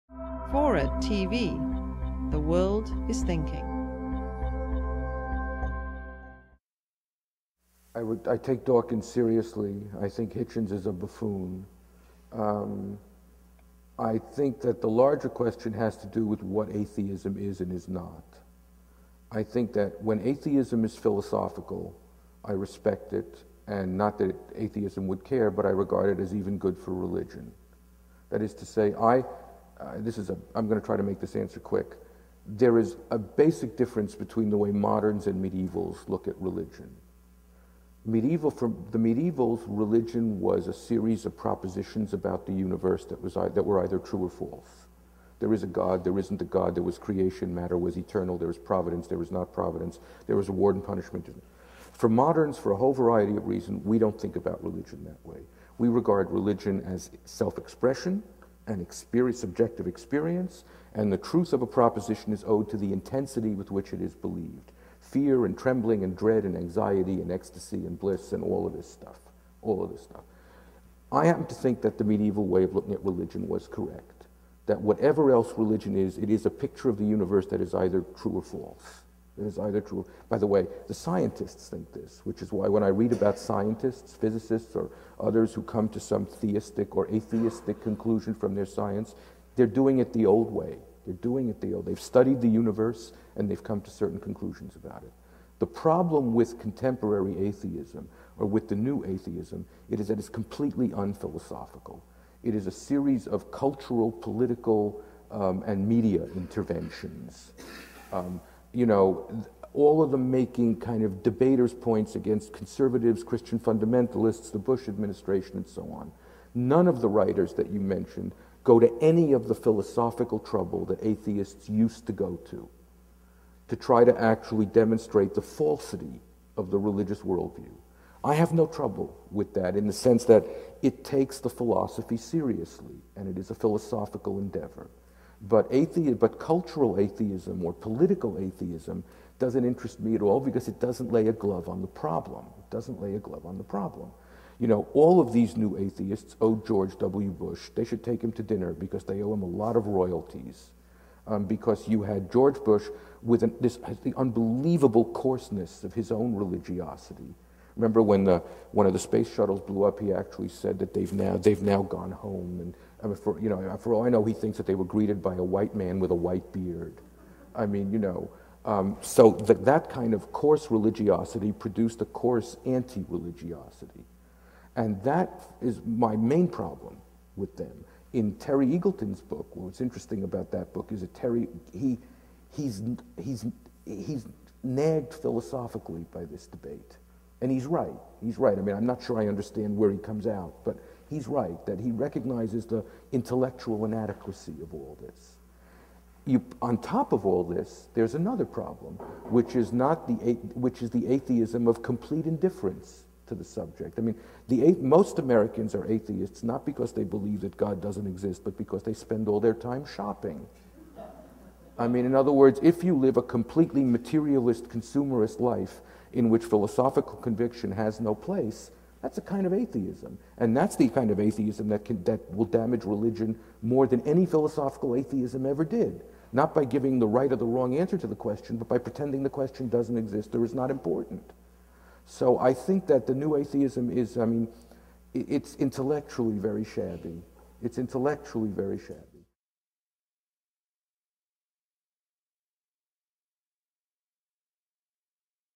TNR Editor Wieseltier Takes On New Atheism, Calls Hitchens a "Buffoon"
Leon Wieseltier, literary editor of The New Republic, weighs in on modern atheism, which he describes as a result of the "coarse" religiosity practiced by evangelicals and strongly religious public figures like George W. Bush. "I take Dawkins seriously," he says. "I think Hitchens is a buffoon."

For over 25 years, Leon Wieseltier has been the literary editor of The New Republic. In that capacity, he has worked with some of the leading writers of our time.

He regularly pens TNR's Washington Diary column and has established himself as one of the most important and erudite critics at work today. He is also the author of the widely acclaimed Jewish theological rumination Kaddish. - CUNY

Leon Wieseltier is a American writer, critic, and magazine editor. Since 1983 he has been the literary editor of The New Republic. Wieseltier was born in Brooklyn, New York and attended the Yeshivah of Flatbush, Columbia University, Oxford University, and Harvard University, and was a member of Harvard's Society of Fellows from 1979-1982. Wieseltier has published several fictional and non-fictional books. Kaddish, a National Book Award finalist in 2000, is a genre-blending meditation on the Jewish prayers of mourning. Against Identity is a collection of thoughts about the modern notion of identity.